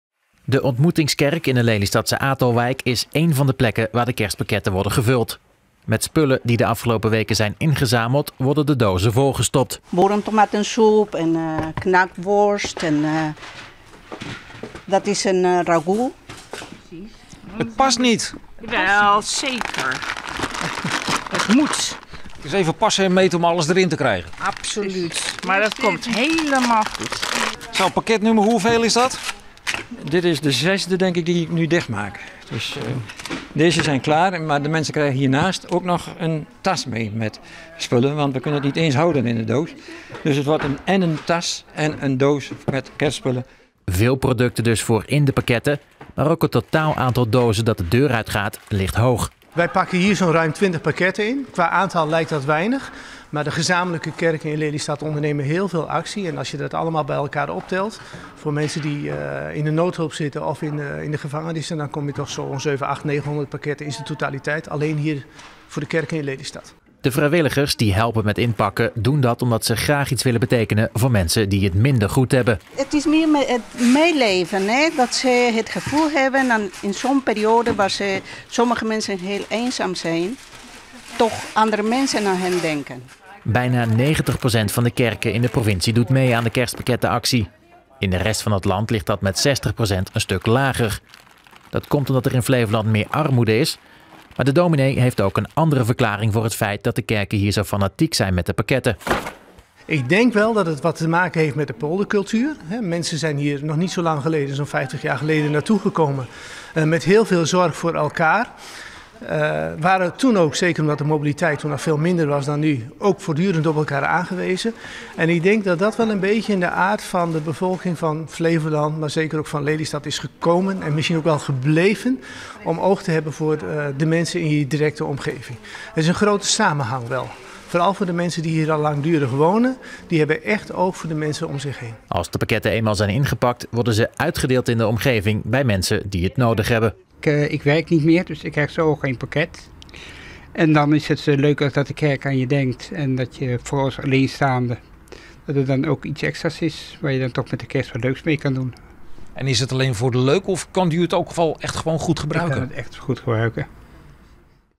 Vrijwilligers Lelystadse kerk druk met kerstpakketten
Vrijwilligers van de Ontmoetingskerk in Lelystad zijn druk bezig met het inpakken en het uitdelen van kerstpakketten voor mensen die het minder goed hebben. Daarin zitten levensmiddelen als boerentomatensoep, knakworsten, eieren en ragout. Onlangs bleek uit onderzoek dat 90 procent van de kerken in onze provincie meedoet aan een kerstpakkettenactie. Dat is opvallend veel, in de rest van het land ligt dat met 60 procent een stuk lager.